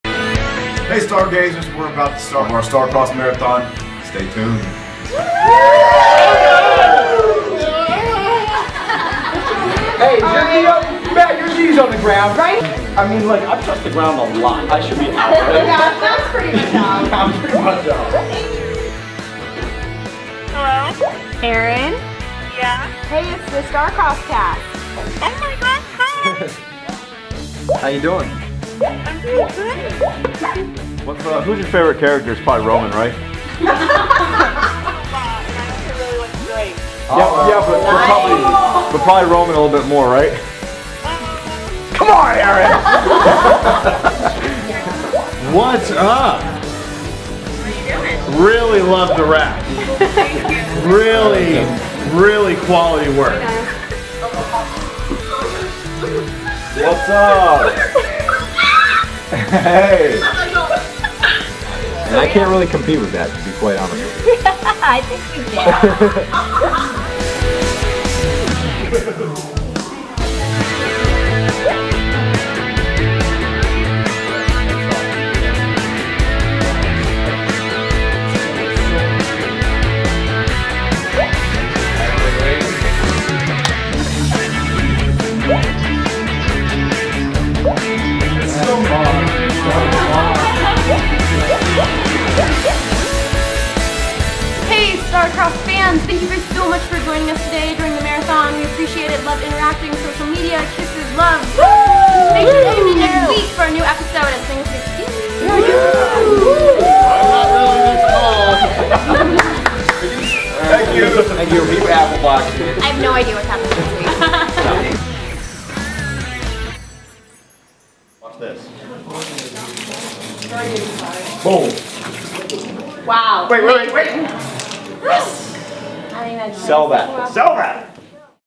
The CW Star-Crossed Marathon Monday
Miss Star-Crossed Marathon Monday or just want to relive it?! Check out the behind-the-scenes fun-filled, 6-hour marathon with the cast of Star-Crossed including Matt Lanter, Aimee Teegarden, Grey Damon, Greg Finley, Natalie Hall, Chelsea Gilligan, Malese Jow, Jesse Luken, Victoria Platt, Johnathon Schaech, Show Creator Meredith Averill, and Show Runner Adele Lim. The fan-centered event featured surprise phone calls to fans (CallMeStarCrossed), Google + Hangouts, live tweeting, Star-Crossed Twister, cast-made T-Shirts, and plenty of laughs!